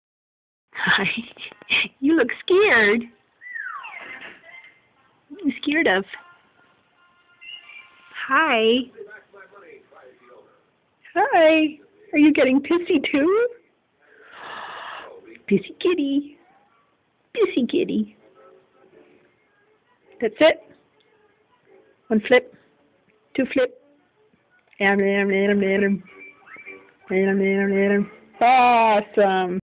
Gitzy the small head cat looking scared and really cute!
My cat is a goof and was lying on her back rolling back and forth. I cut the video clip short when she started molesting herself. So proud!